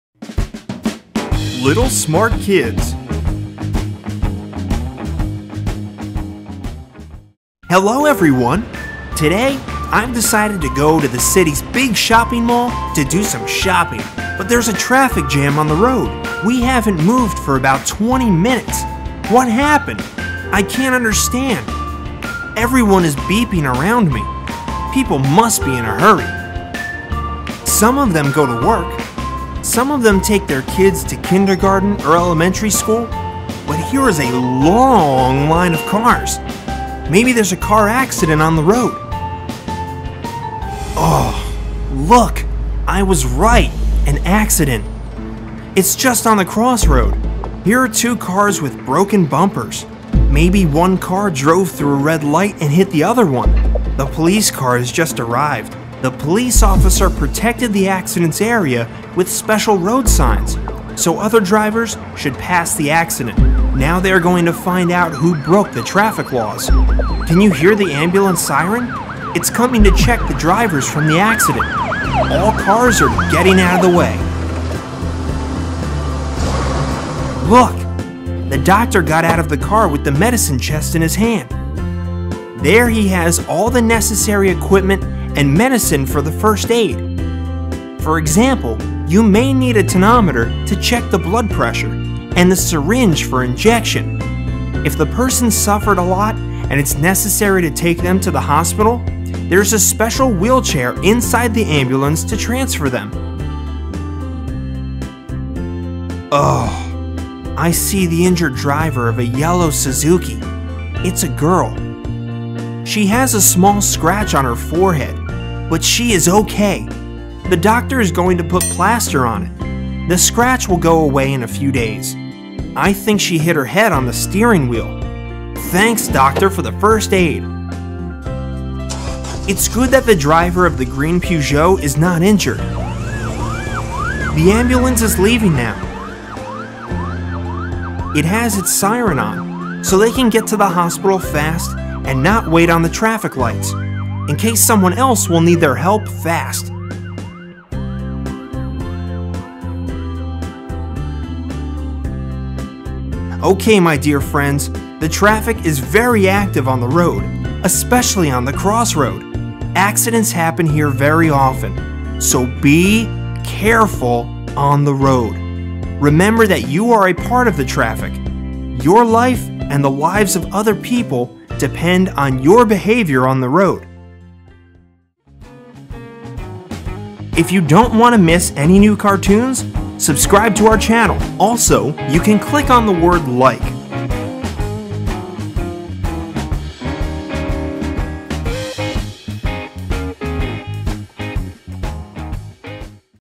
Ambulance For Kids Helps.. - Little Smart Kids
Today Handy Andy rode around the city and there is,.
Handy Andy called an ambulance as soon as he saw the accident. In this cartoon kids will see how ambulance helps people.